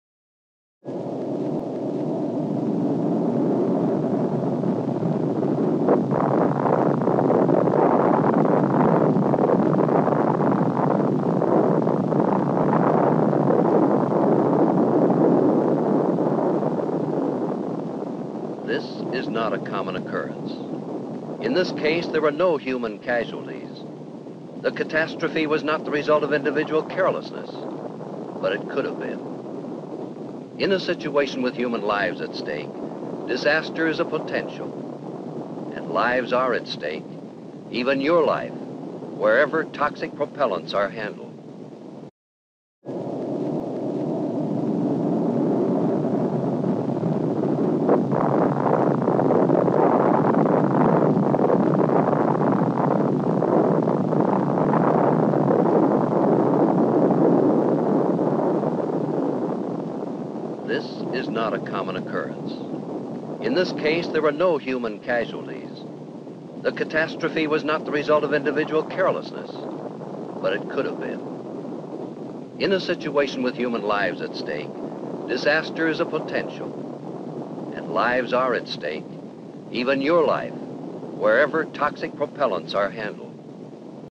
Atlas-Centaur Rocket Explodes On Pad - 1965 Rocket Launch Failure - WDTVLIVE42
The United States has suffered many failures during its attempts to launch rockets with some rather spectacular explosions being the result. Here we see the 2 .

Atlas-Centaur 5 suffered a sudden main fuel valve closure at T+0.88 seconds during its launch from Cape Canaveral LC 36A on March 2, 1965. The rocket fell .

1960's Atlas Centaur Rocket Explodes On The Launch Pad.